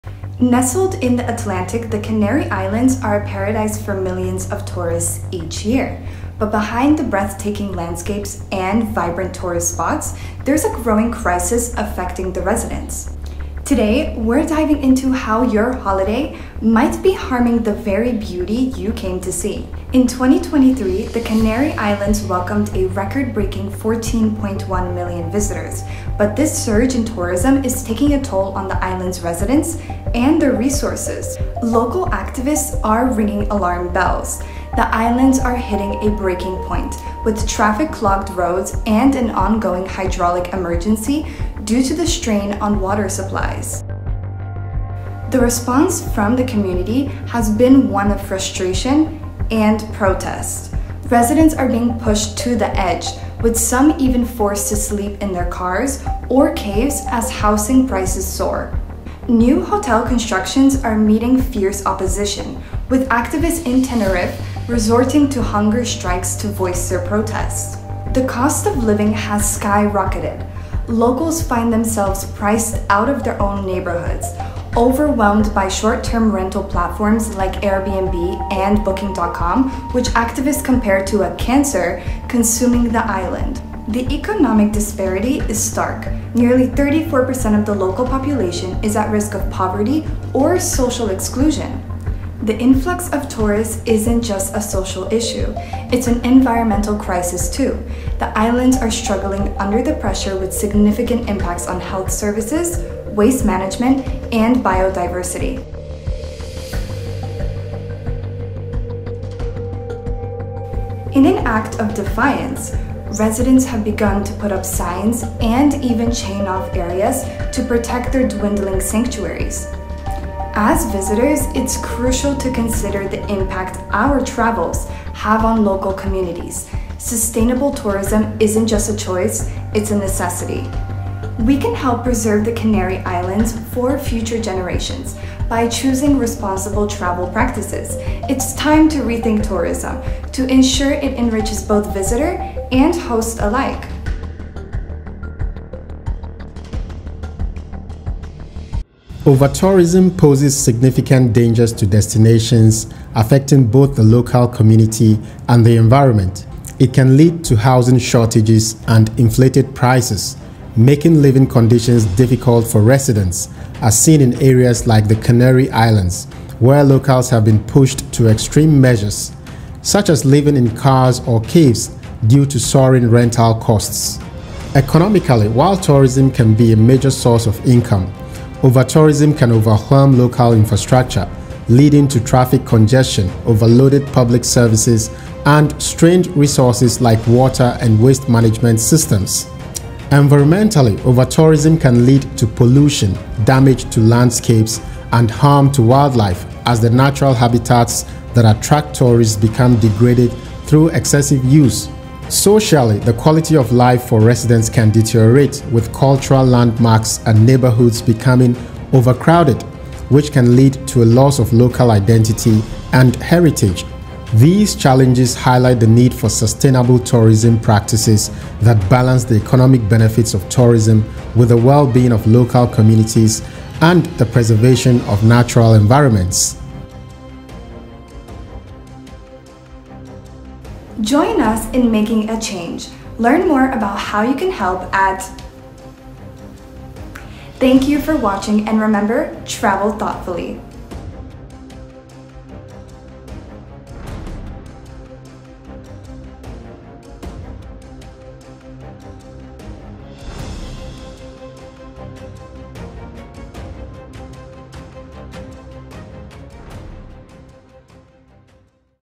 Tourists Go Home: The Dark Side of Vacationing in Canary Islands!
Discover the unseen side of the Canary Islands in this eye-opening documentary. While the islands are a paradise for millions, their popularity has a dark side impacting the locals. In "Tourists Go Home: The Harsh Reality Behind Canary Islands' Tourism Boom," we explore the dire consequences of overtourism faced by the residents.

What's Inside:

Overwhelming Visitor Impact: In 2023 alone, the Canary Islands saw a record-breaking 14.1 million visitors. Dive deep into how this surge is straining local resources, from water supplies to housing.

Local Backlash: Witness the community's frustration, as overcrowded living conditions and skyrocketing housing prices push residents to their limits. See firsthand accounts of protests and opposition against new hotel constructions.

Economic Disparities: Learn about the challenges locals face, with nearly 34% at risk of poverty. Understand the role of short-term rental platforms in exacerbating these issues.

Environmental Urgency: Tourist influx isn't just a social issue—it's also an environmental crisis. From overloaded health services to biodiversity threats, discover the significant impacts.

Sustainable Solutions: Hear about actions being taken to promote sustainable tourism. See inspiring examples of responsible travel that helps preserve the beauty and community of the Canary Islands.

Join the Conversation:
Become part of the solution. Visit [insert sustainable tourism resource] to learn how your travel choices can support sustainable tourism and help maintain the Canary Islands for future generations.